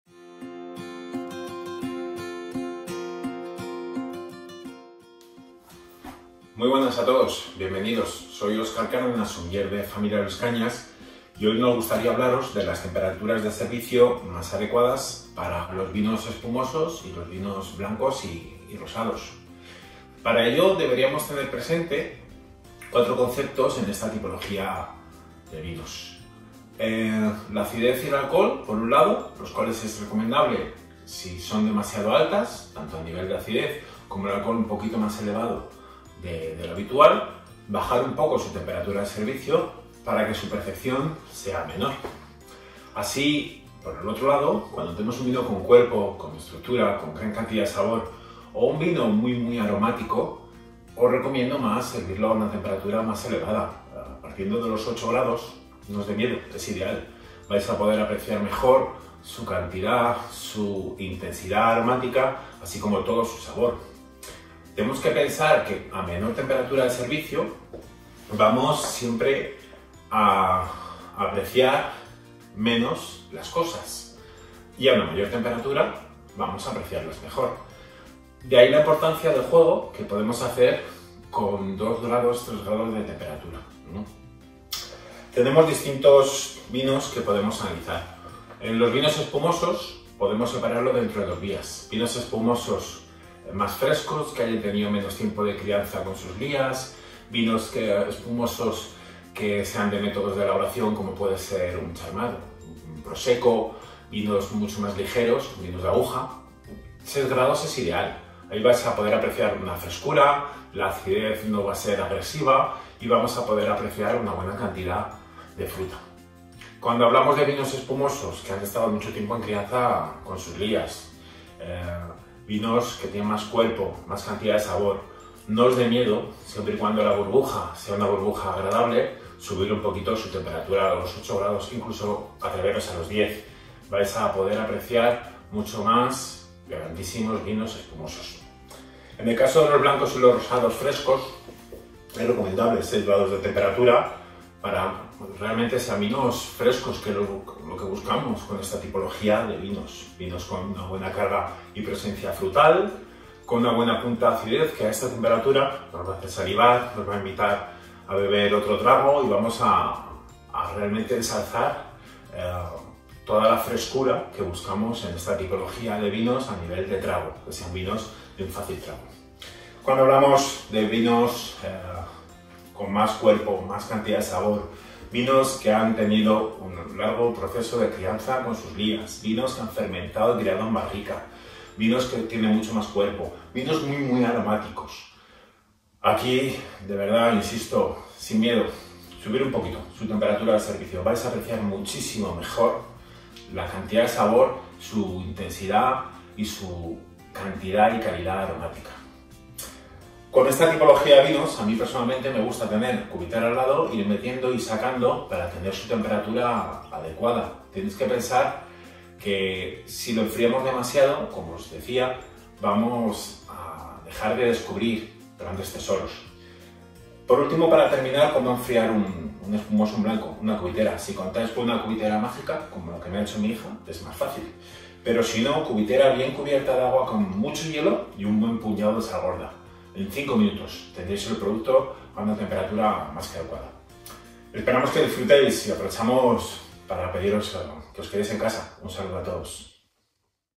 VINEXPERT. Vol 3: Temperaturas de servicio en vinos espumosos y blancos
Seguimos conectados con nuestros alumnos a través de estos vídeos hechosencasa!!
En este vídeo nuestro sumiller Oscar Cárdenas nos explica la importancia de las temperaturas de servicio en los vinos espumosos y blancos y qué factores influyen.